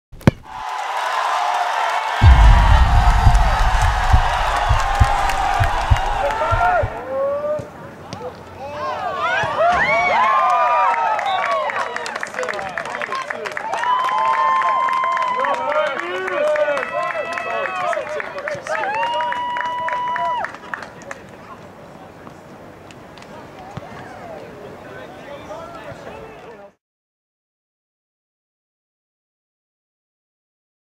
2012 JL Mann Soccer - Goal Oelsner - Assist Mattern vs Hillcrest
2012 JL Mann Soccer - Goal Christian Oelsner - Assist Connor Mattern vs Hillcrest